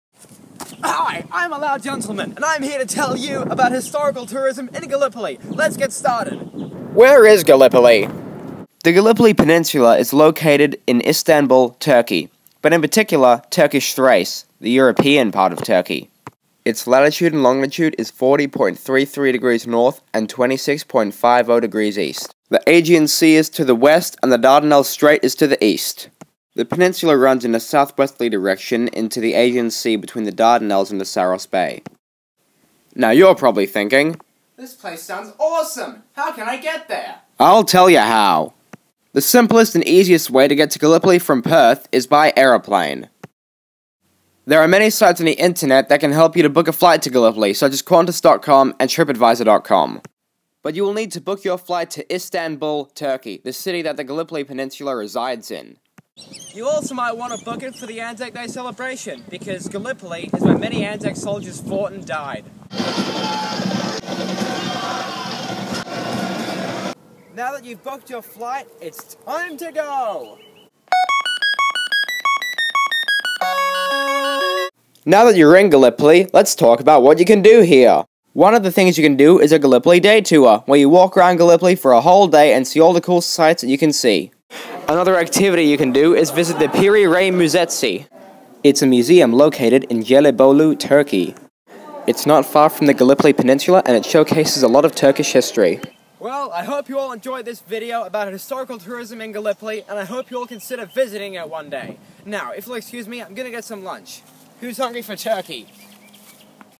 Historical Tourism In Gallipoli
This is a video that me and two of my friends made for Humanities class explaining historical tourism in Gallipoli, Turkey.